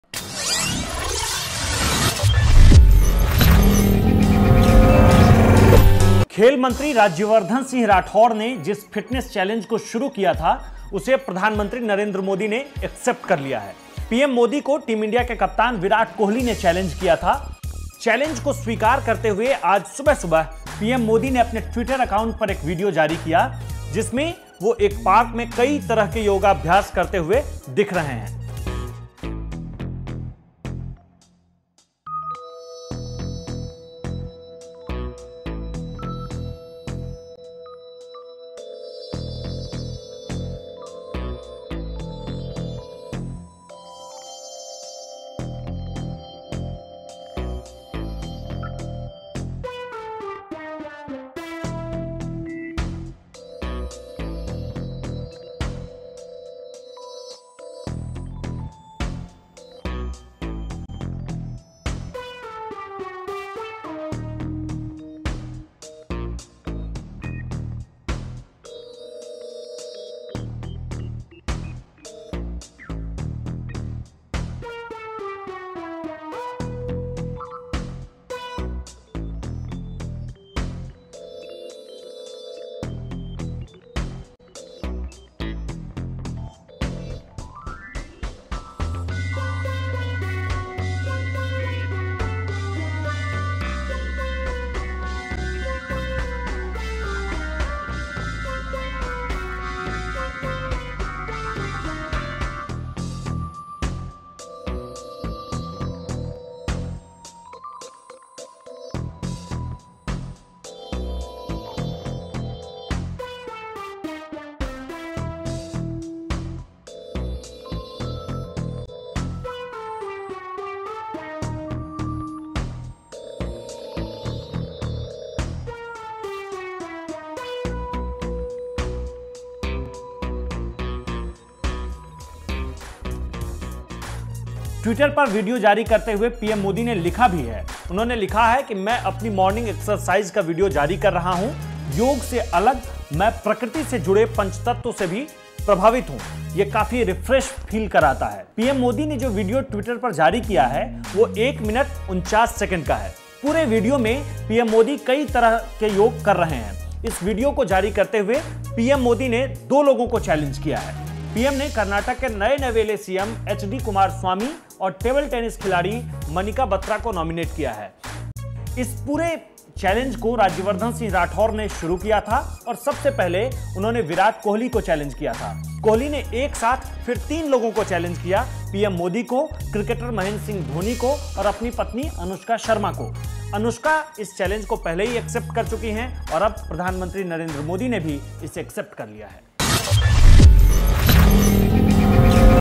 Virat के Fitness Challenge का PM Modi ने ऐसा दिया जवाब
PM Modi ने Virat Kohli के Fitness Challenge को Accept कर लिया है. पीएम मोदी के फिटनेस वीडियो को लेकर देखिए हमारी खास रिपोर्ट.